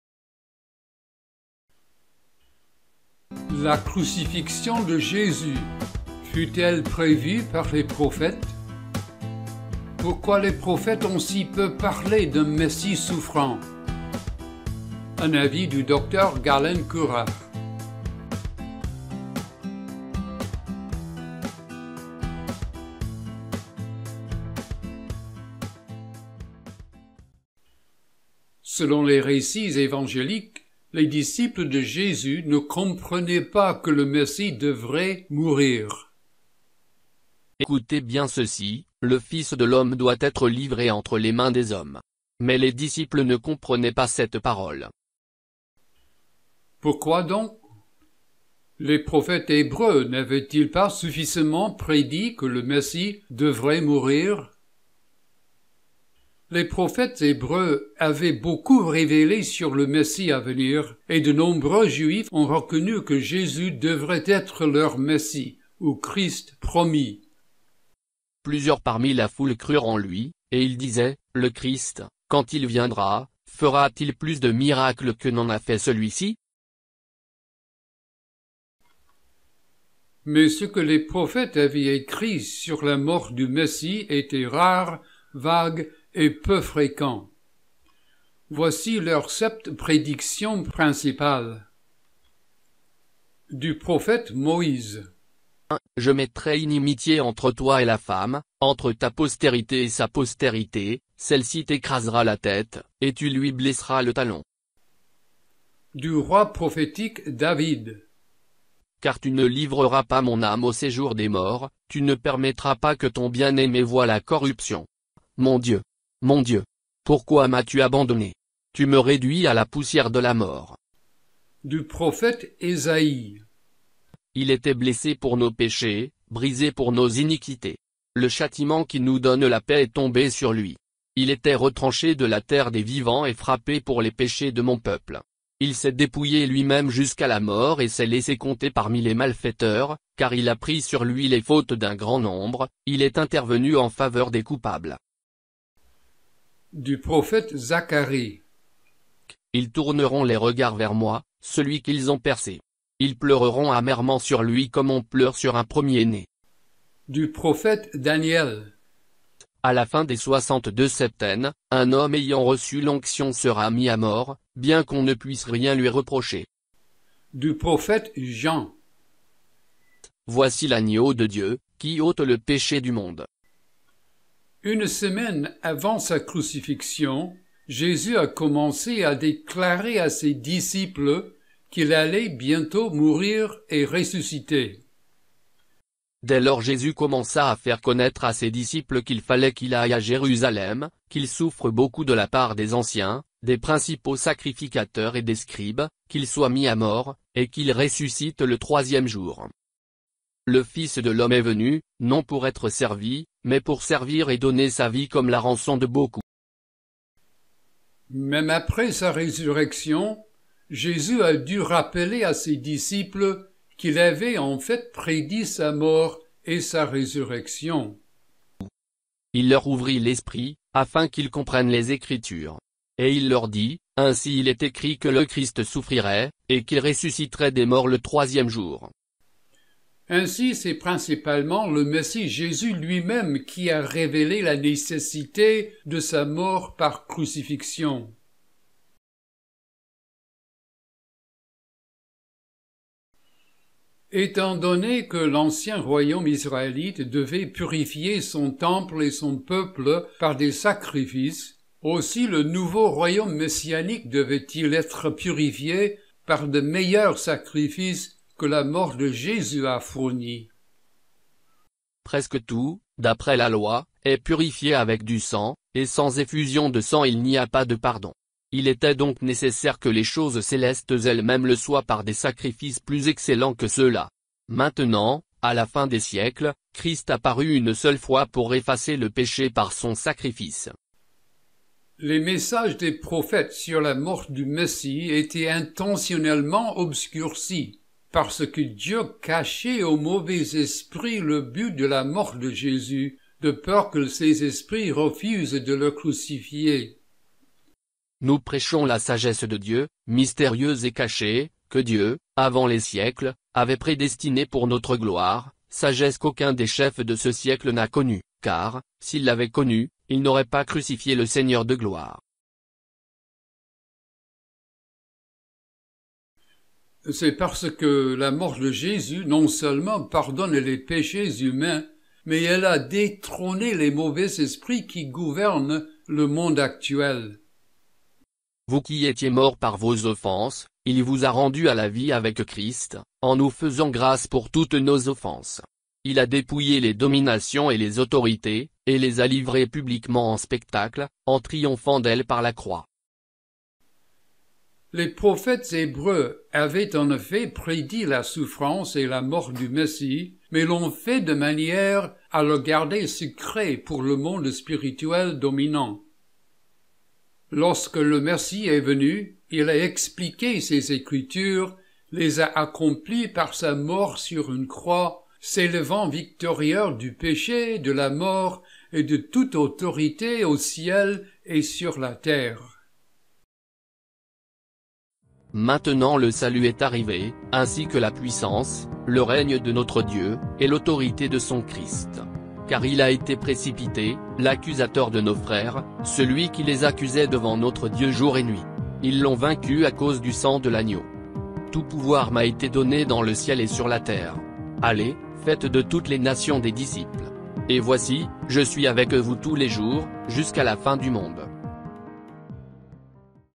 La crucifixion de Jésus fut-elle prévue par les prophètes ?
Pourquoi les prophètes ont si peu parlé d'un Messie souffrant
Un avis du Dr Galen Currah, juillet 2020
Citations bibliques tirées de la Nouvelle édition de Genève (1997)
wordwatchers.currah.us

Introduction

Selon les récits évangéliques, les disciples de Jésus ne comprenaient pas que le Messie devrait mourir.

Luc 9 : 44-45

Pourquoi donc? Les prophètes hébreux n'avaient-ils pas suffisamment prédit que le Messie devrait mourir?

Les prophètes hébreux avaient beaucoup révélé sur le Messie à venir, et de nombreux Juifs ont reconnu que Jésus devait être leur Messie (ou Christ) promis.

Les prédictions

Mais ce que les prophètes avaient écrit sur la mort du Messie était rare, vague et peu fréquent. Voici leurs sept prédictions principales.

Le prophète Moïse (15e siècle avant notre ère)

(1) Genèse 3 : 15

Le roi prophétique David (10e siècle avant notre ère)

(2) Psaume 16 : 10

(3) Psaume 22 : 2, 16

Le prophète Ésaïe (8e siècle avant notre ère)

(4) Ésaïe 53 : 5, 8, 12

Le prophète Zacharie (6e siècle avant notre ère)

(5) Zacharie 12 : 10

Le prophète Daniel (5e siècle avant notre ère)

(6) Daniel 9 : 26

Le prophète Jean (1er siècle de notre ère)

(7) Jean 1 : 29

Une semaine avant sa crucifixion, Jésus a commencé à déclarer à ses disciples qu'il allait bientôt mourir et ressusciter.

Matthieu 16 : 21 ; Luc 9 : 22

Marc 10 : 45

Même après sa résurrection, Jésus a dû rappeler à ses disciples qu'il avait en fait prédit sa mort et sa résurrection.

Ainsi, c'est principalement le Messie Jésus lui-même qui a révélé la nécessité de sa mort par crucifixion.

Leur sens

Étant donné que l'ancien royaume israélite devait purifier son temple et son peuple par des sacrifices, aussi le nouveau royaume messianique devait-il être purifier par de meilleurs sacrifices que la mort de Jésus a fournis.

Hébreux 9 : 22-26

Les messages des prophètes sur la mort du Messie étaient intentionnellement obscurcis, parce que Dieu cachait aux mauvais esprits le but de la mort de Jésus, de peur que ces esprits refusent de le crucifier.

1 Corinthiens 2 : 7-8

C'est parce que la mort de Jésus non seulement pardonne les péchés humains, mais elle a détrôné les mauvais esprits qui gouvernent le monde actuel.

Colossiens 2 : 13-15

Conclusion

Les prophètes hébreux avaient en effet prédit la souffrance et la mort du Messie, mais l'ont fait de manière à le garder secret pour le monde spirituel dominant.

Lorsque le Messie est venu, il a expliqué ces Écritures, les a accomplies par sa mort sur une croix, s'élevant victorieux du péché, de la mort et de toute autorité au ciel et sur la terre.

Apocalypse 12:10-11